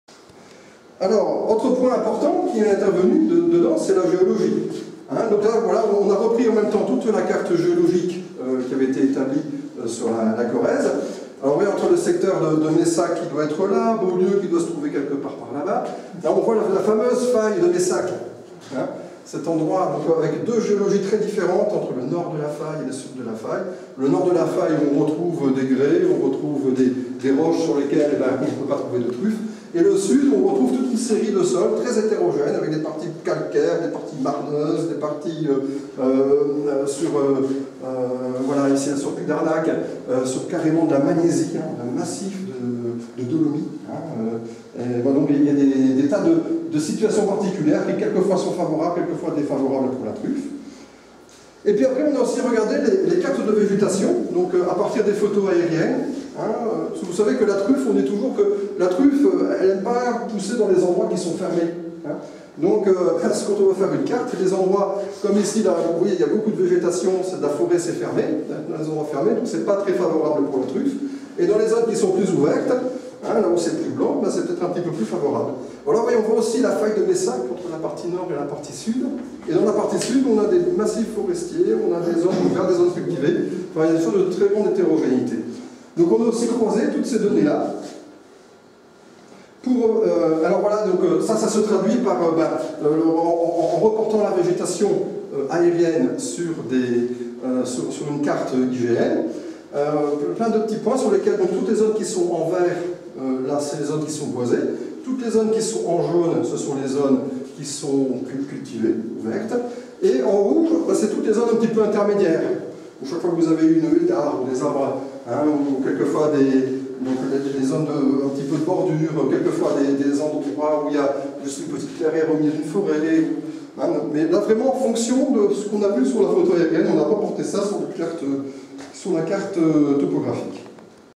TRUFFES (carto 2) : portes ouvertes station Chartrier Ferrière le 6 juin 2012
Hervé COVES présente la cartograhie des sols truffiers - Source ADIDA - CDA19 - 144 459